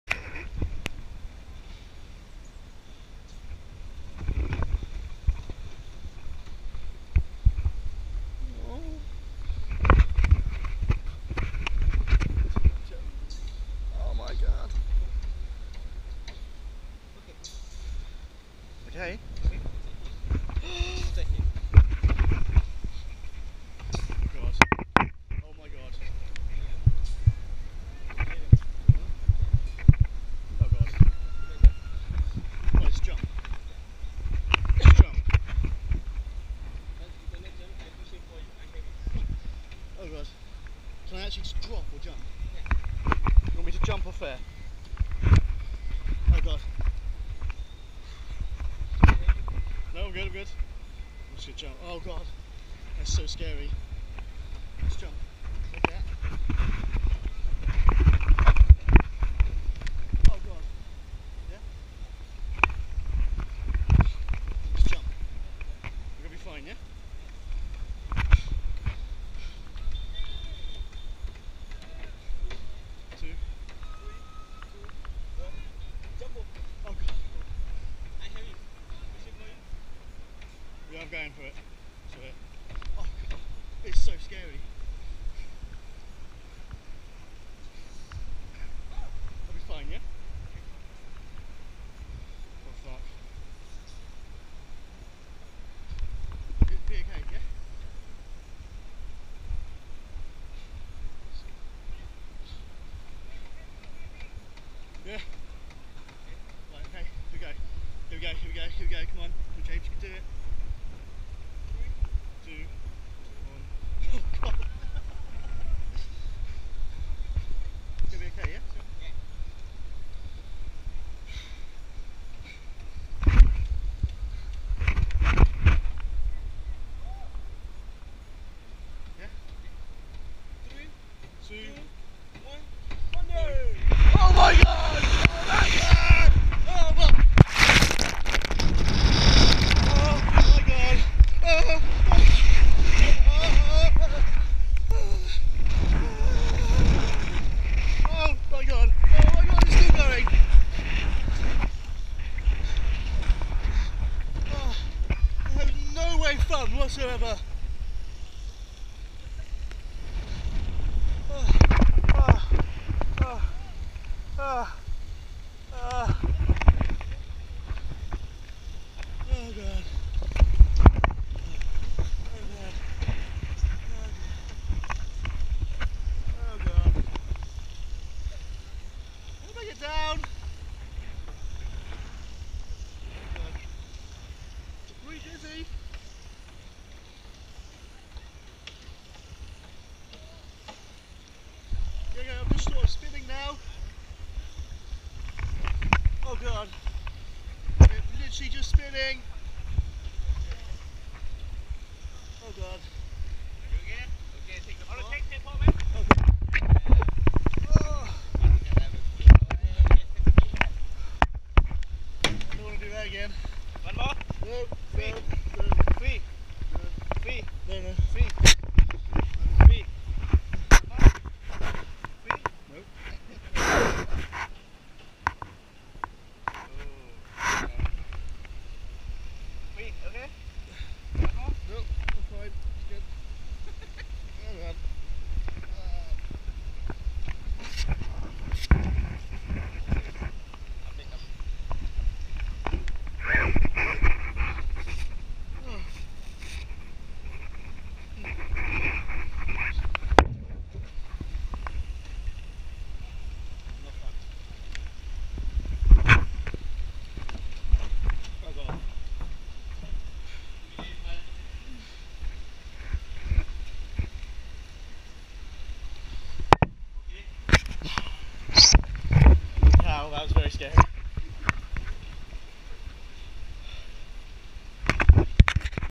Bungee Jump, Phuket
Never again! Was horrible, did not enjoy it but this is me bottling it for a few mins then eventually jumping! All filmed on my GoPro Camera